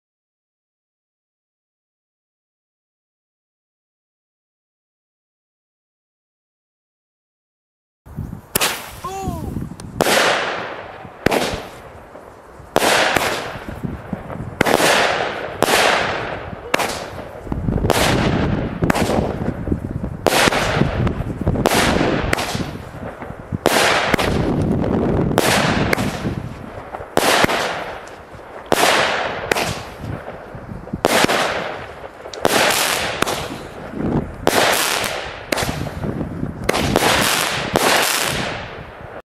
NICO Rhino Feuerwerksbatterie
Kleine Feuerwerksbatterie mit 16 Schuss Kategorie F2